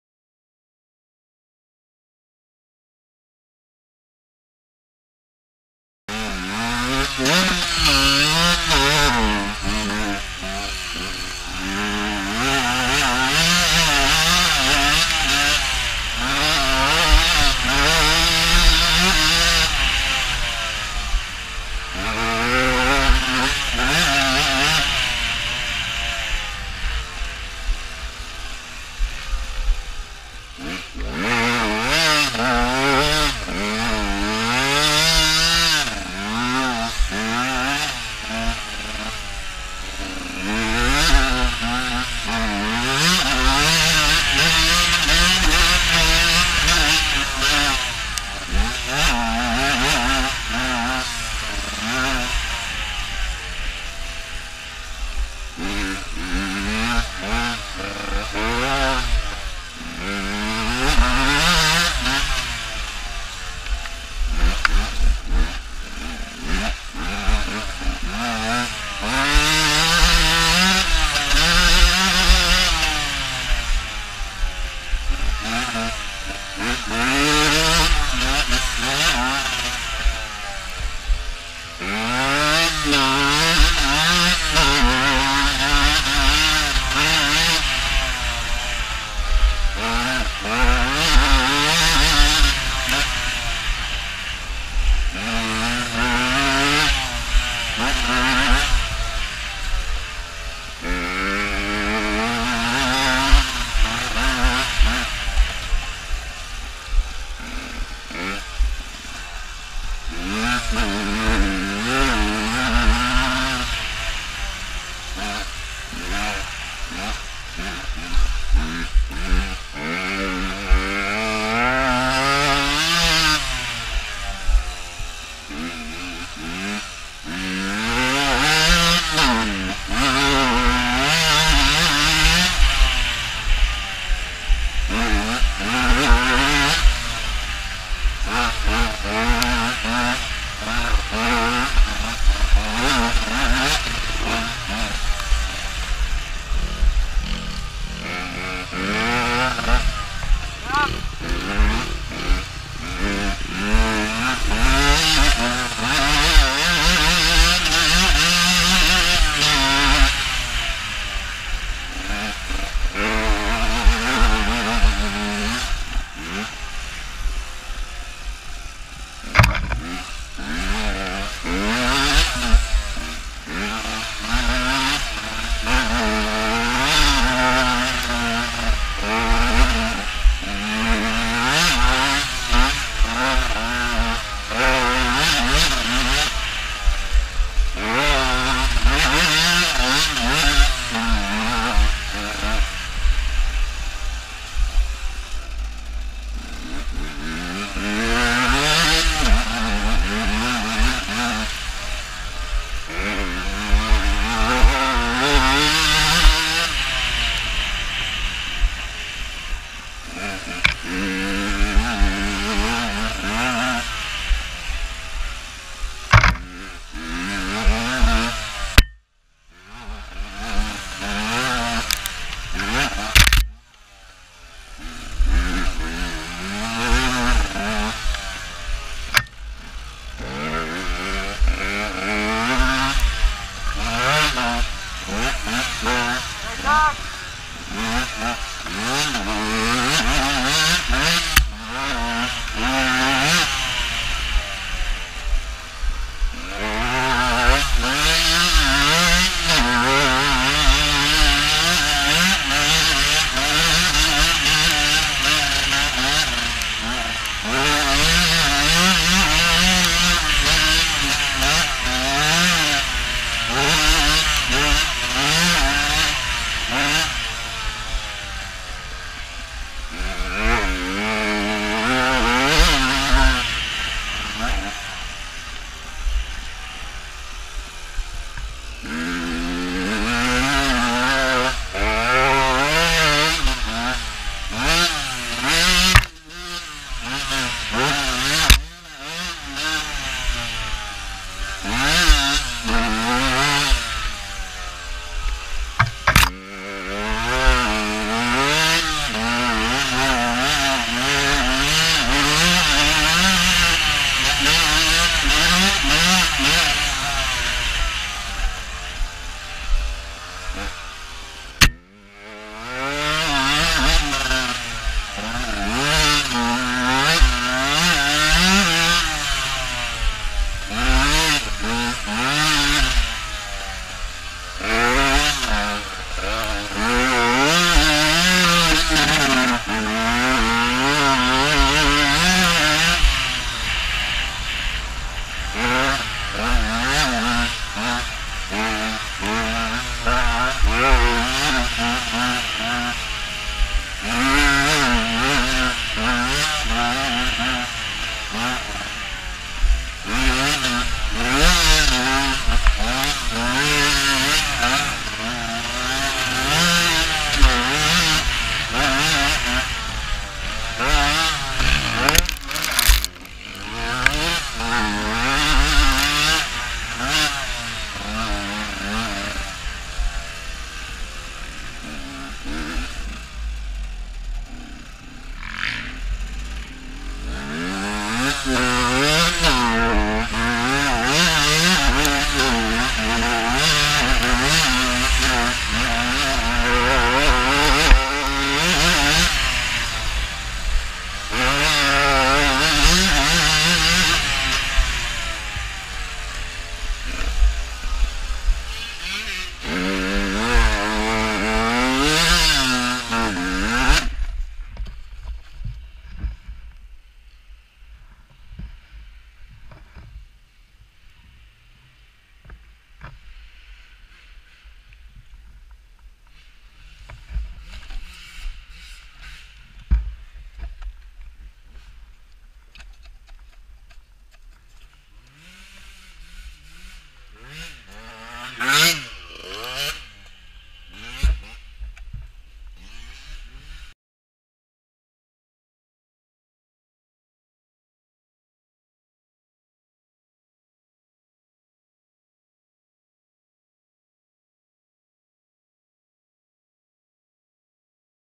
Motionscupen Delt 3 Göta ms
Hojen gick väldigt konstigt hela tävlingen som man märker mycket i
början me tillslut vänjde jag mig. Så första varvet 7.30 och snabbaste
5.50.